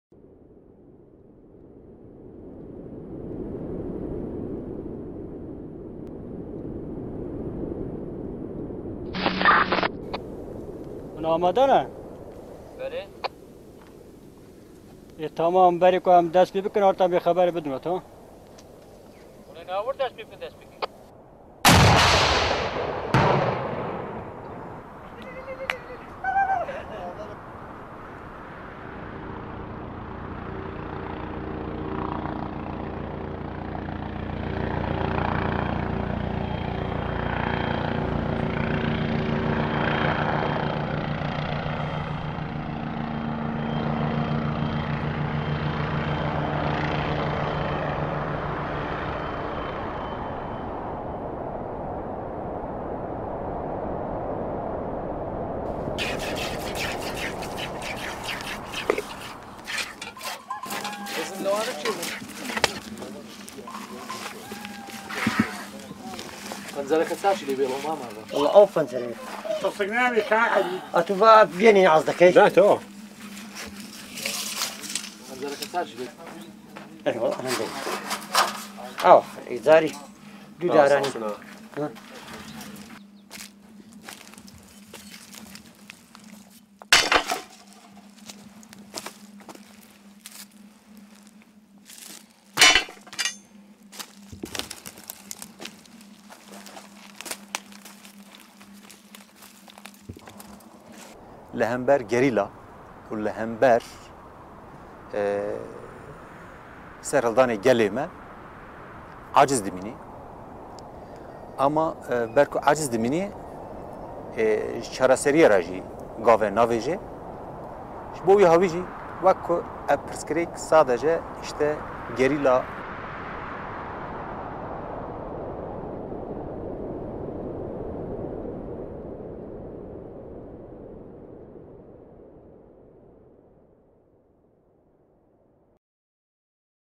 Kandil mendiak sutan / Qandil mountains on fire (Diaporama)
PKKko gerlariak ez ezik, Kandil mendietako herritar xumeak turkiar ejerzioaren xede bilakatu dira. Horiexek dira Ankarak deituriko "mugaz gaindiko operazioak": sekretuak bezain bortitzak.

Argazkiak:
Karlos Zurutuza, Borja Portuondo, Najiba Muhamad, Turkiako ejertzitoa.

Audioa: Borja Portuondo

Edizioa: Lander Arbelaitz